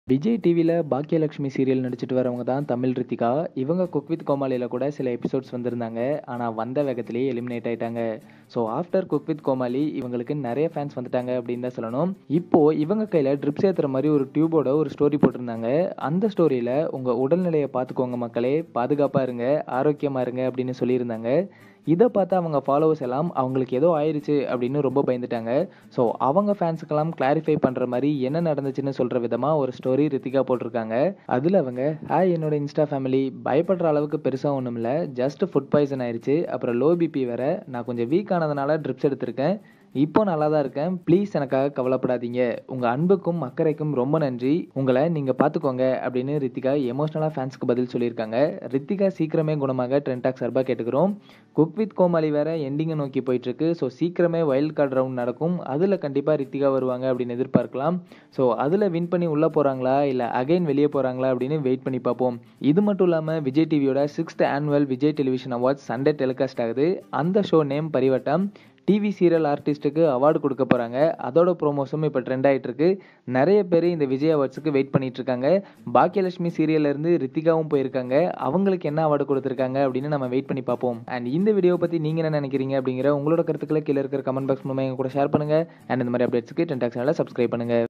Cook With Comali Rithika Hospitalized😢 | BakiyaLakshmi serial Amirtha, Vijay Bala
Created by tea class entertainment team

shere